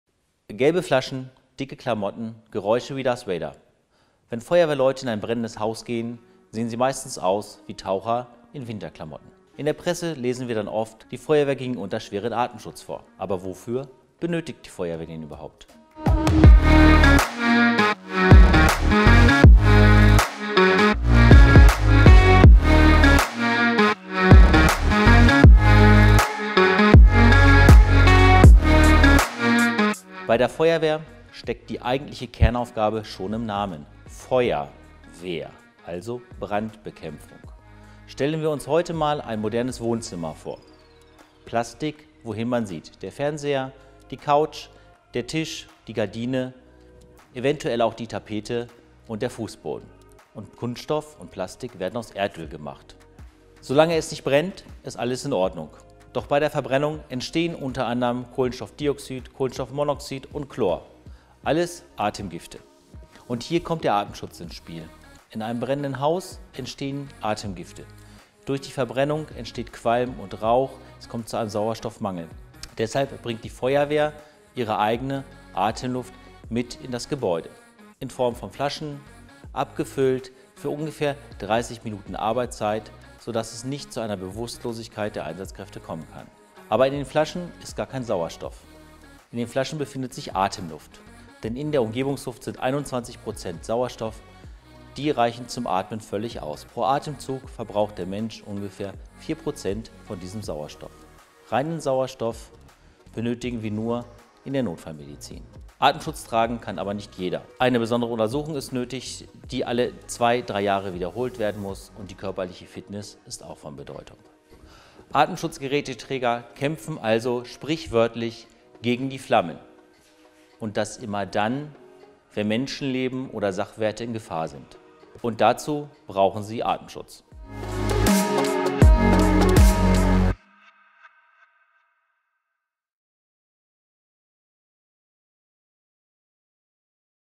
Wofür braucht die Feuerwehr Atemschutz?
Wenn es brennt, dann ist oft von "schwerem Atemschutz" die Rede. In der dritten Staffel von "Feuerwehr erklärt in 112 Sekunden" klären wir heute mit Florian Peters die Frage, wozu diese gelben Flaschen auf dem Rücken eigentlich gebraucht werden.